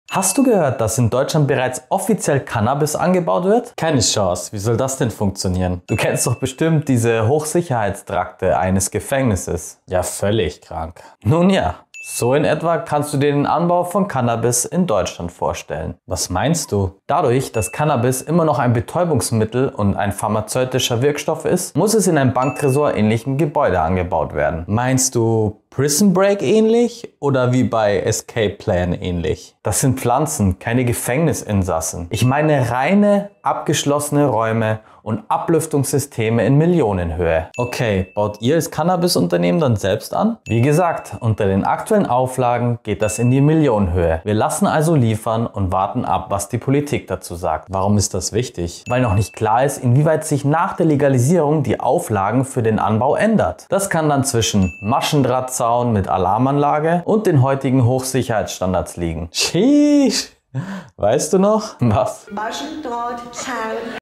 Bubatz-Anbau hinter dicken Mauern
synbioticerklärt die Sicherheitsbedingungen unter denen Cannabis aktuell in Deutschland offiziell angebaut wird. Hast du gewusst, dass die Bestimmungen so umfangreich sind?